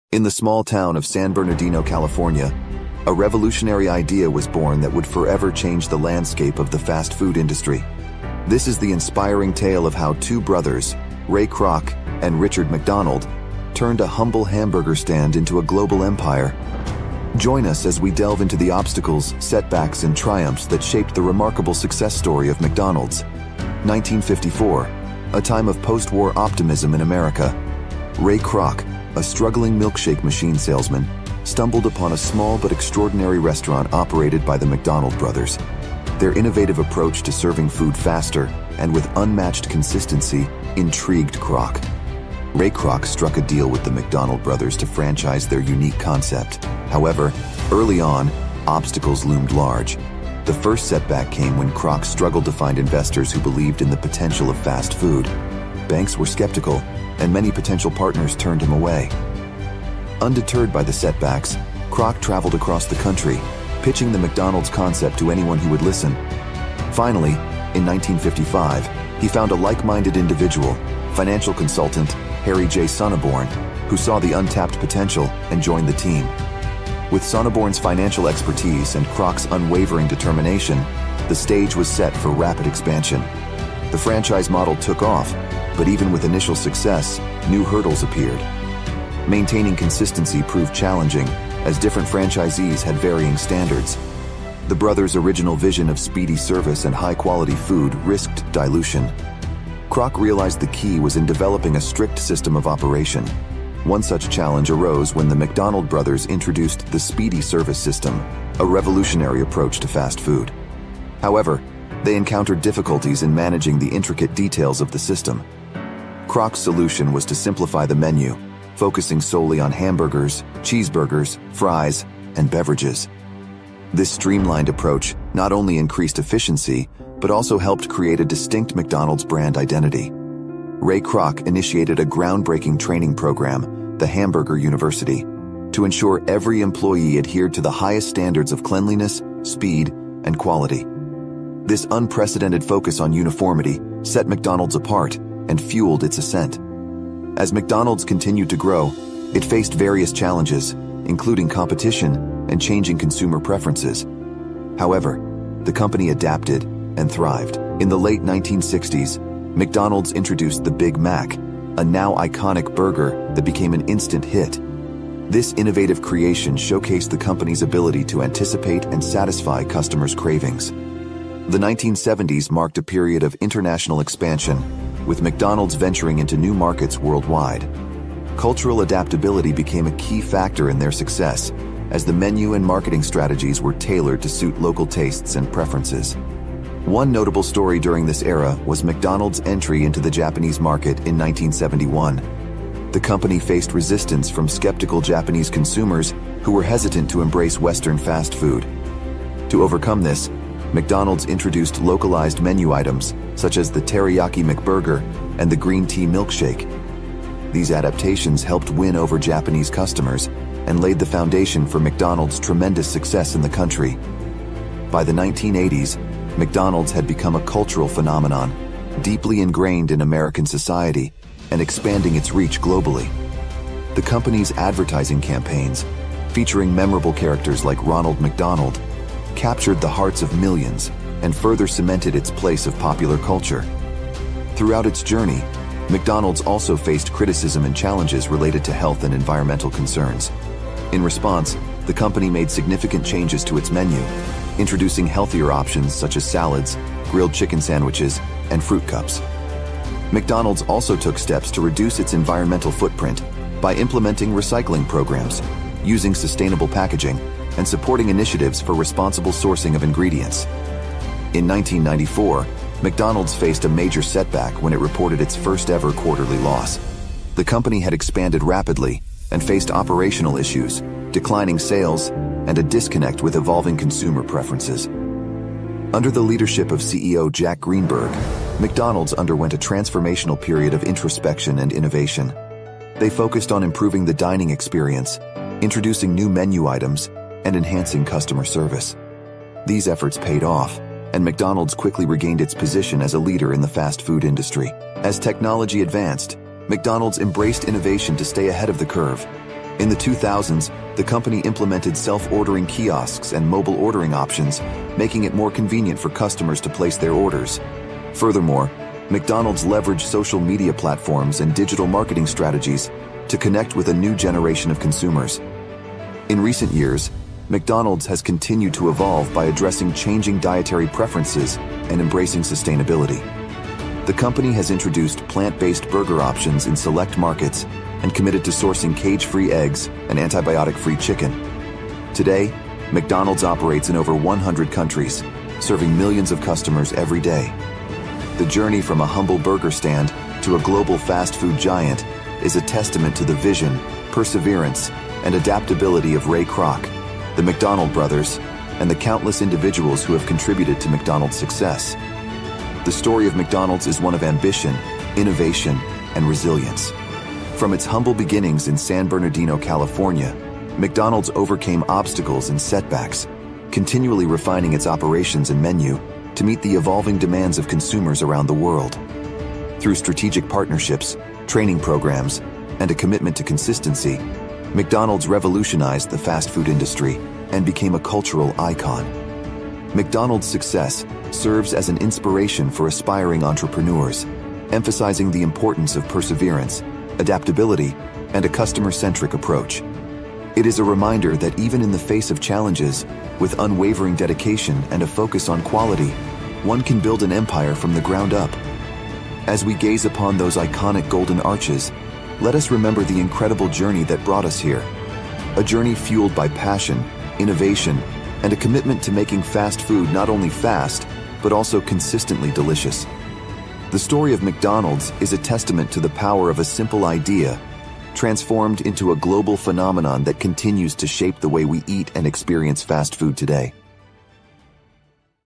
How McDonald's Became The MOST PROFITABLE Fast Food Chain In The World! $200 BILLION SUCCESS STORY!
Ray Kroc and Richard McDonald, transformed a humble Hamburger Stand into a global empire in the small town of San Bernardino, California.
From Ray Kroc's chance encounter with the innovative McDonald Brothers to the challenges of finding investors who believed in the potential of fast food, witness the unwavering determination and visionary thinking that propelled McDonald's to unprecedented heights. Experience the evolution of the menu, the introduction of iconic burgers like The Big Mac, and the company's expansion into international markets. Through moments of adaptation, innovation, and a commitment to customer satisfaction, McDonald's became not just a fast food giant, but a cultural phenomenon deeply ingrained in popular culture. This inspiring story highlights the power of perseverance, adaptability, and a customer-centric approach. Get ready to be inspired by the incredible journey that transformed a simple idea into a global sensation.